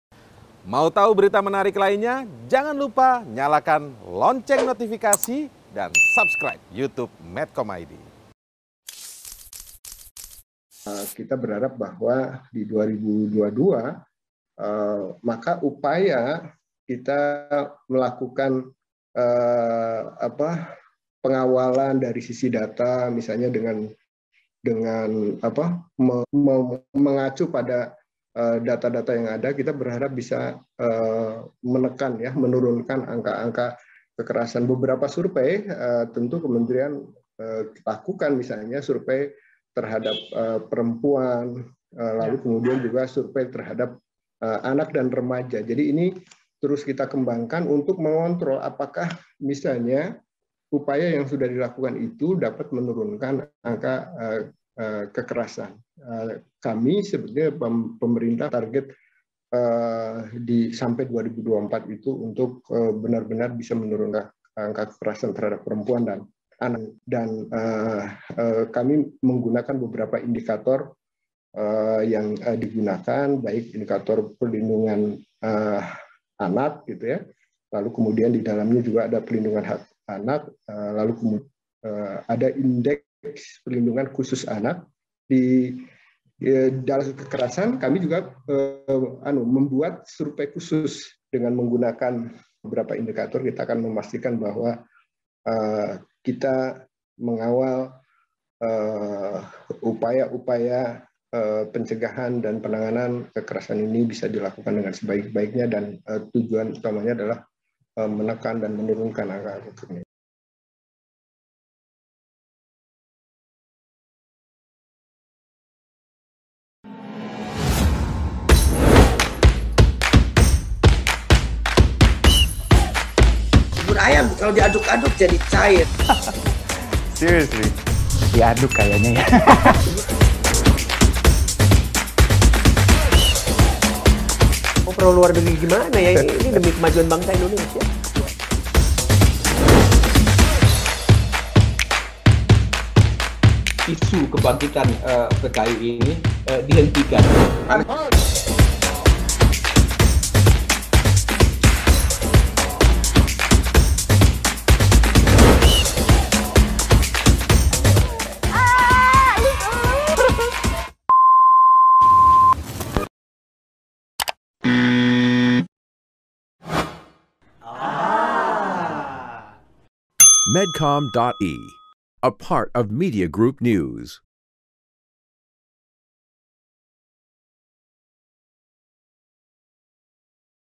Kementerian PPPA: Tahun 2024 Ditargetkan Angka Kekerasan Terhadap Anak Menurun
Pemerintah memiliki target hingga tahun 2024 angka kekerasan terhadap anak menurun. Dengan menggunakan beberapa indikator perlindungan anak yang didalamnya terdapat perlindungan hak anak dan indeks perlindungan khusus anak.

Deputi Perlindung Khusus Anak Kementerian Pemberdayaan Perempuan dan Perlindungan Anak, Nahar, berharap upaya pengawalan data pada tahun 2022 benar-benar dapat menurunkan angka kasus kekerasan terhadap anak.